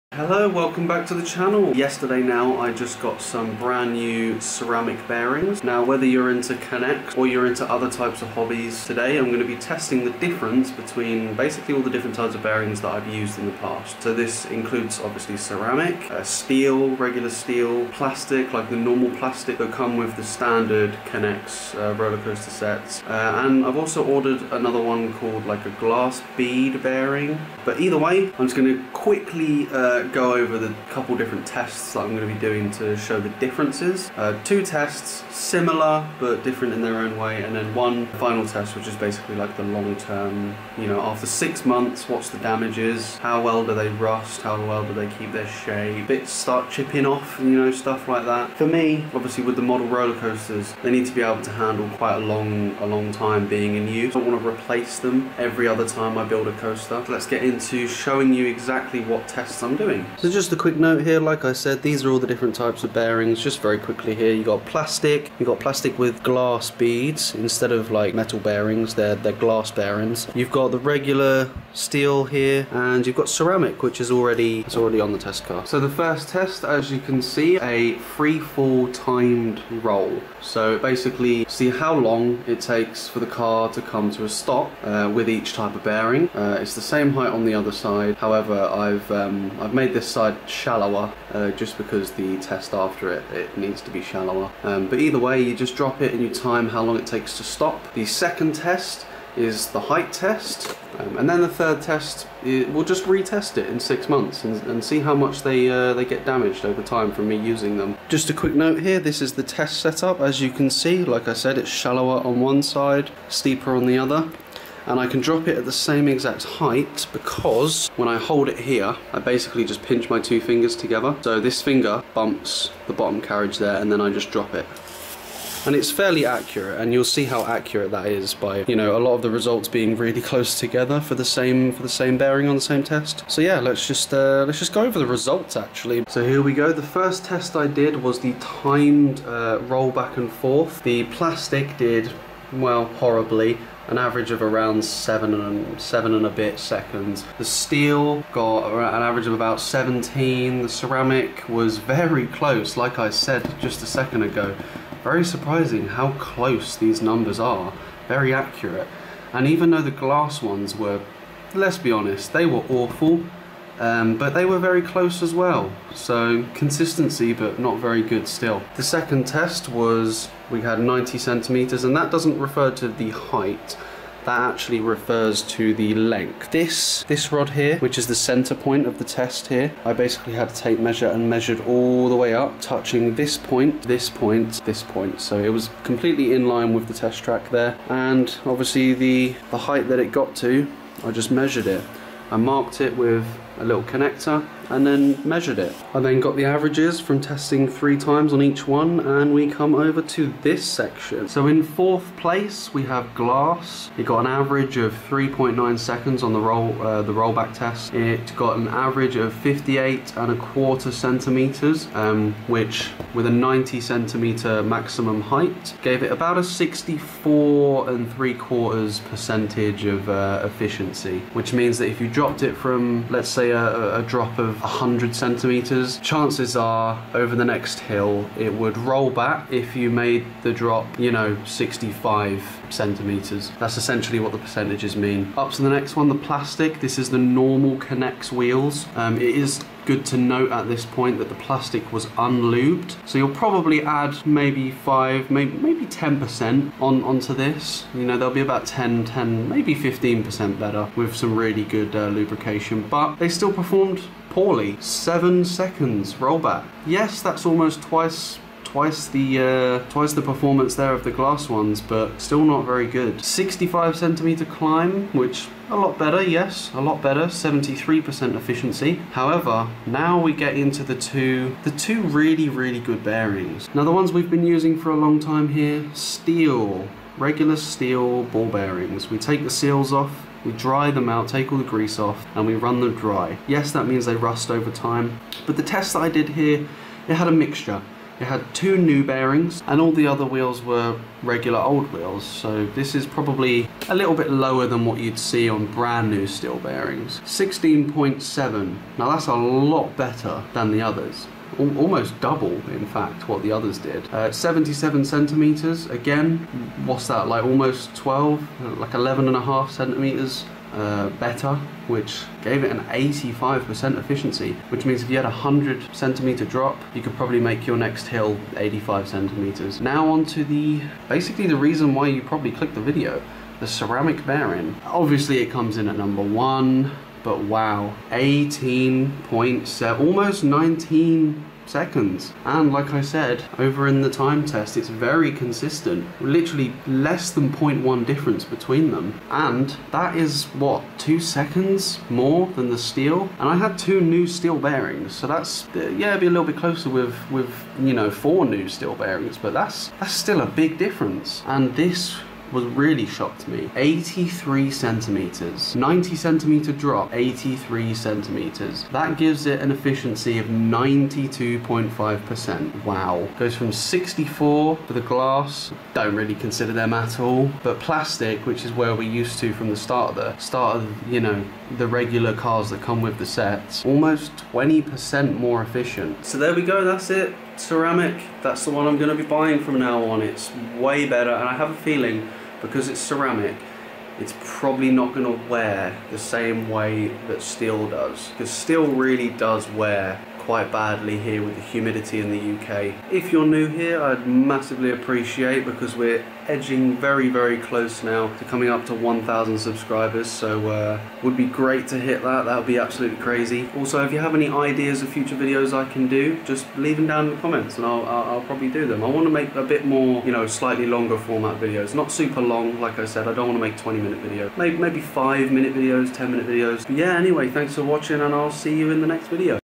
THE SECRET SAUCE FOR ALL KNEX ROLLERCOASTER BUILDS!
I think i've found my new favorite bearings!
Today we test 4 types of bearings: steel, plastic, glass and ceramic to see which one will spin longer!
The test setup is a leangth of knex roller coaster track with a drop and hill.
we will test both how long it takes a roller coaster train with each type of bearing to come to a stop.
we will also test the height at which the car can climb back up the other side.